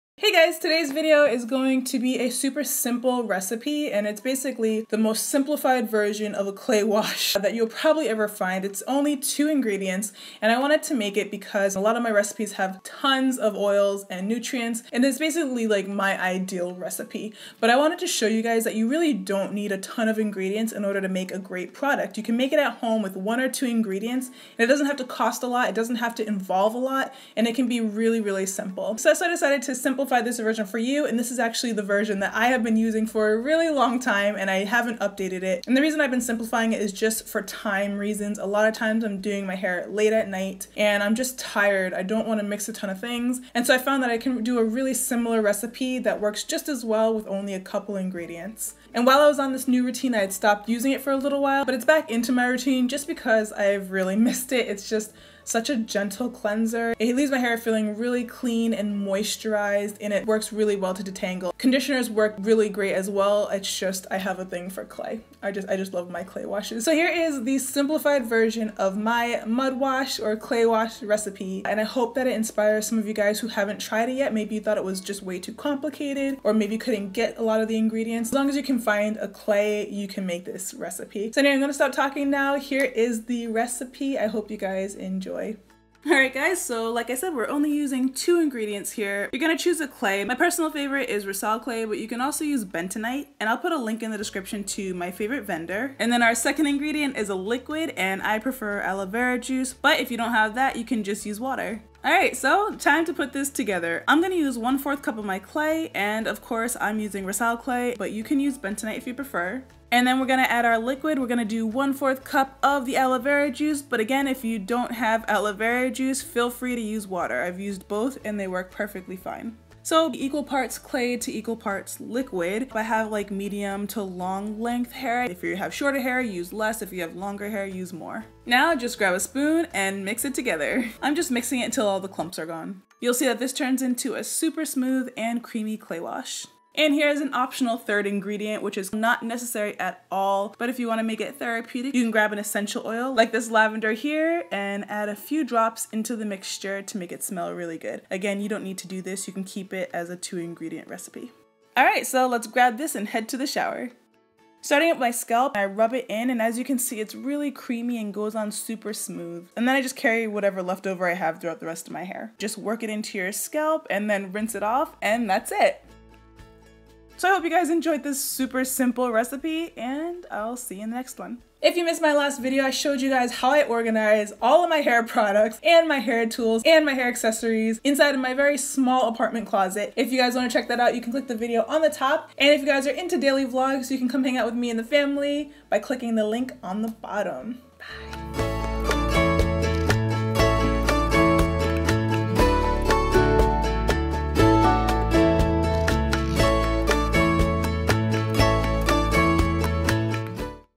Easiest 2 Ingredient Clay Mud Wash Recipe | Simple DIY Hair Care - Naptural85
Hey Guys! Time to take it down a notch... I wanted to show you guys just how simple my homemade recipes can be! Here's the most simple and easiest clay mud wash recipe you'll find on the internet! It can be made in less than a minute, and involves only two ingredients for a creamy, smooth and naturally cleansing clay wash! I hope it helps! XO! Nap